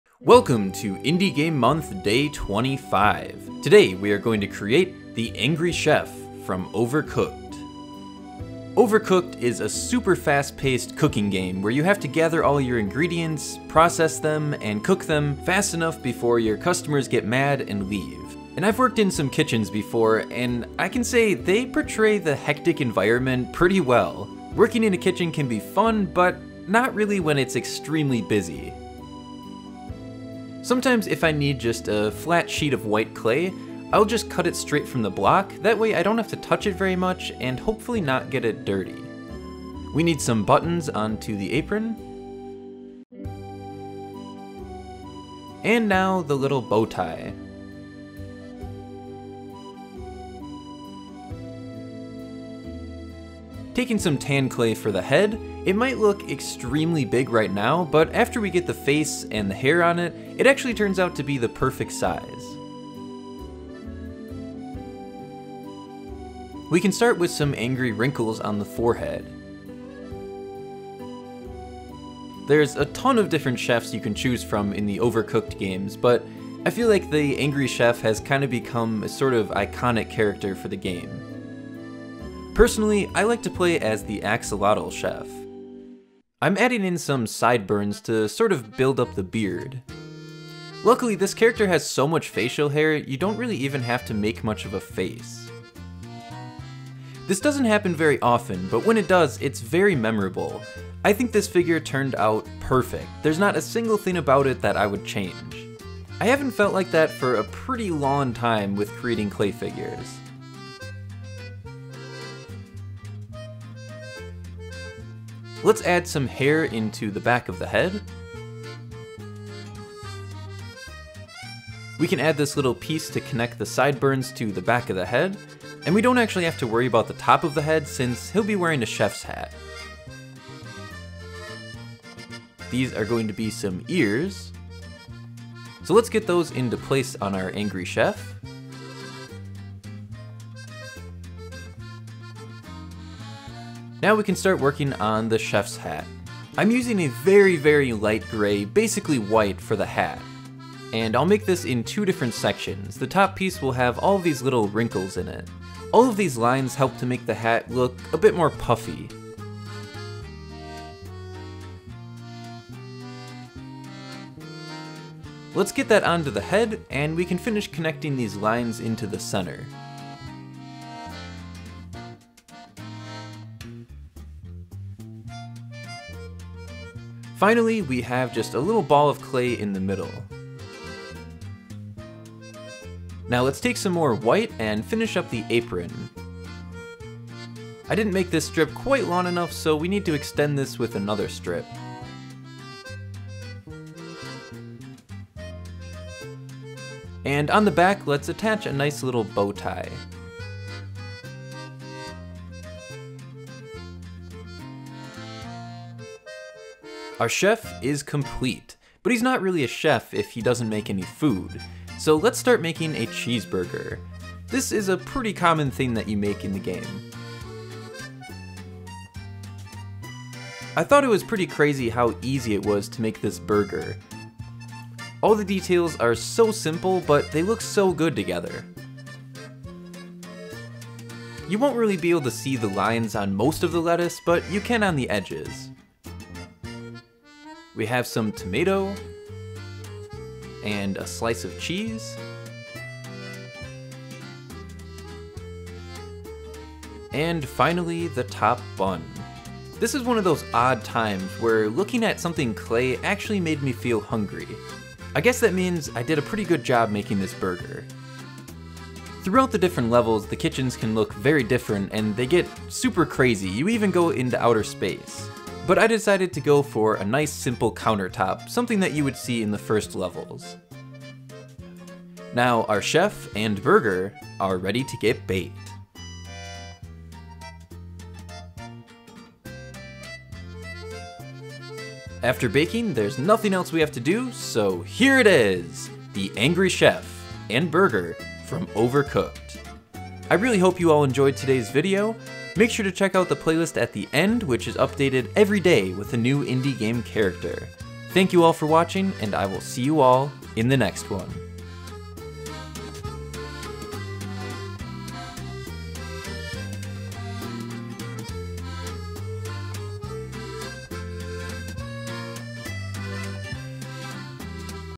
Making The Angry Chef With Polymer Clay (Overcooked 1+2)
Welcome to day 25 of Indie Game Month! Today we are going to create The Angry Chef from Overcooked 1+2! If you want to see a playlist that is updated daily with new characters, you can check it out
I hope you all enjoyed the video! I will see you all tomorrow!
 - The clay I used is FIMO soft polymer clay